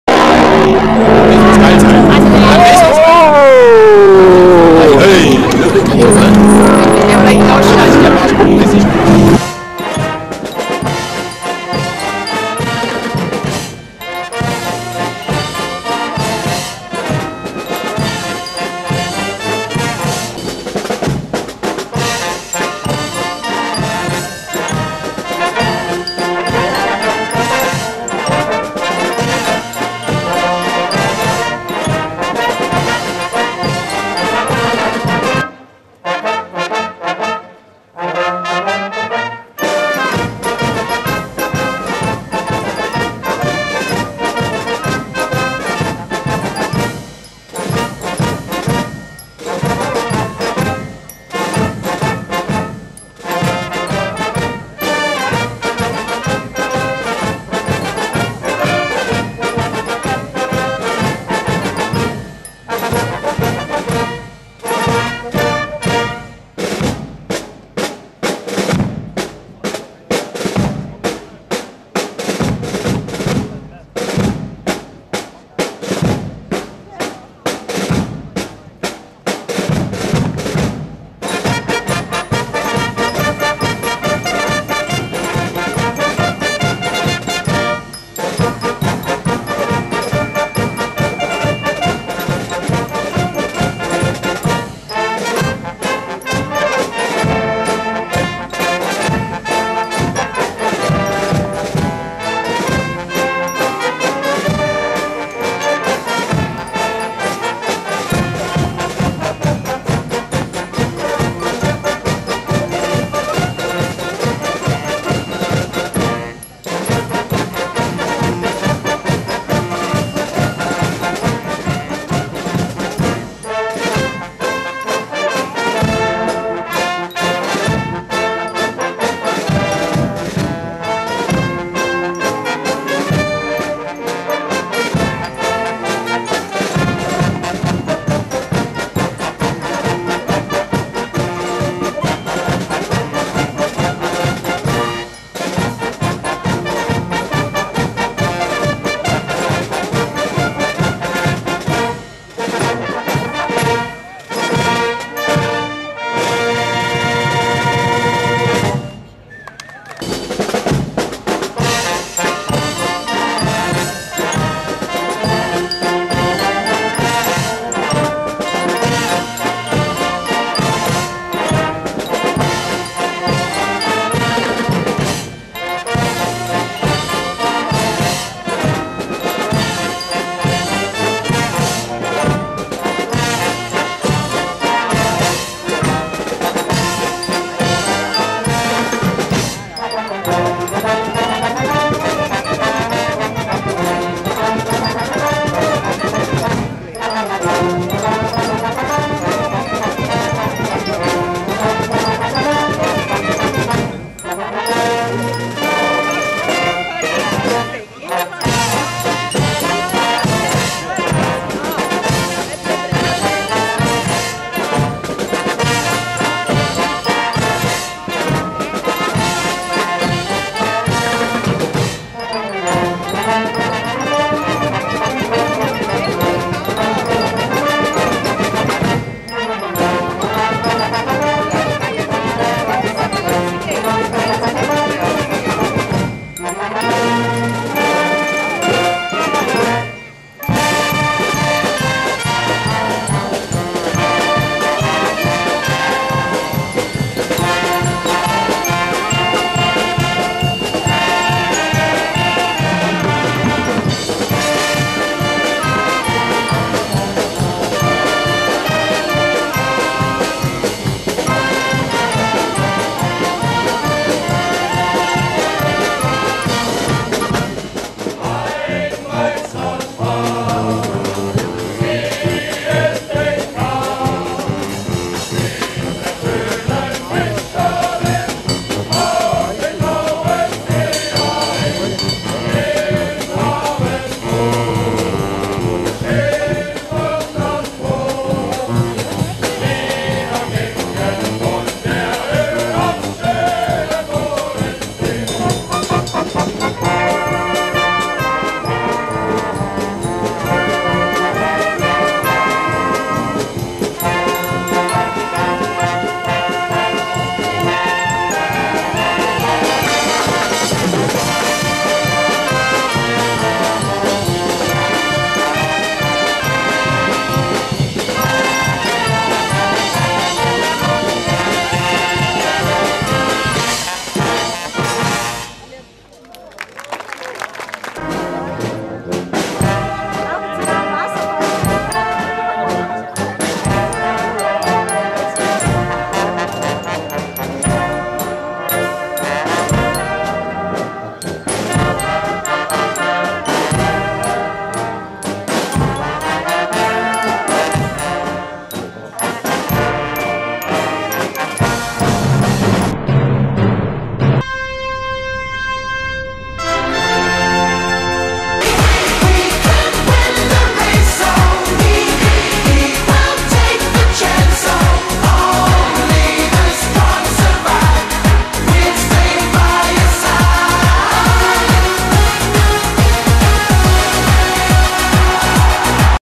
200920521 Väter spielen Musik und Kinder spenden Bier am Vatertag in HD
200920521 Väter spielen Musik und Kinder spenden Bier am Vatertag am Bodensee in Friedrichshafen in HD Fathers play music and children donate beer on Father's Day at Lake Constance